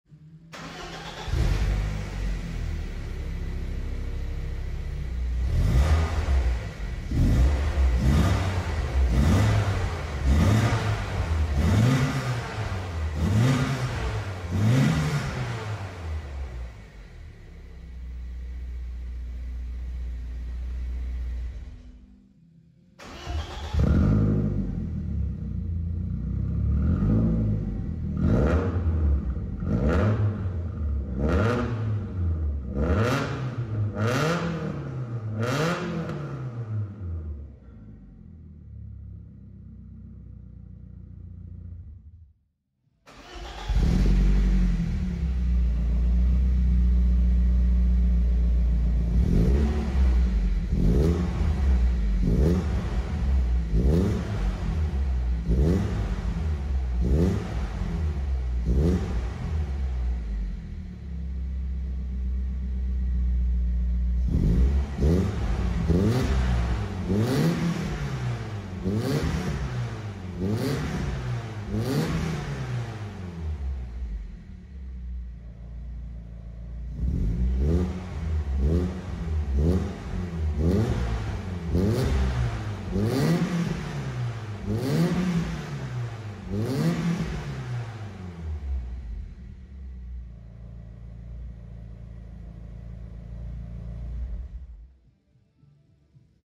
BMW 318iS E30 Exhaust Sound
Der universal Endschalldämpfer stammt von Ebay. Das Endrohr beträgt 70mm und musste seperat gelängt sowie geschweisst werden. Die Abgasanlage ist ab Katalysator neu gebaut inklusive dem originalen Mittelschalldämpfer. Erstmal heiss fahren und schauen wie sich der ESD klang technisch „verbessert“.. sonst wird der MSD bald durch ein weiteres Rohr ersetzt!!!